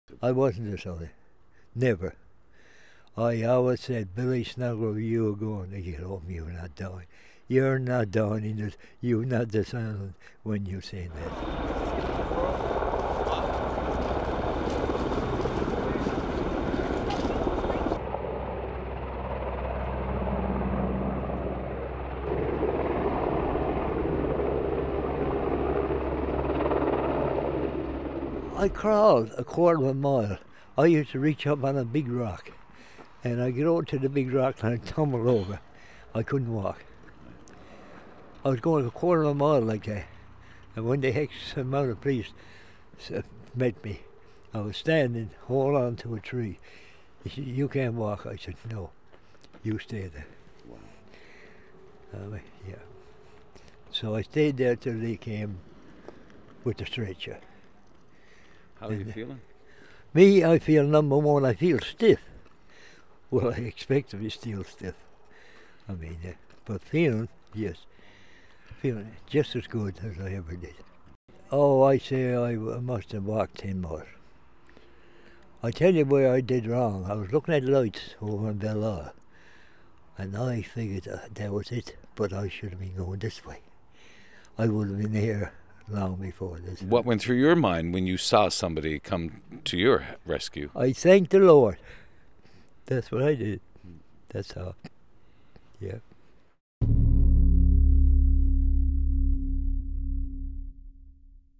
82-year-old grateful for rescue
Billy Snelgrove says he thanked God when he saw rescuers coming his way after spending more than 24 hours lost in the woods.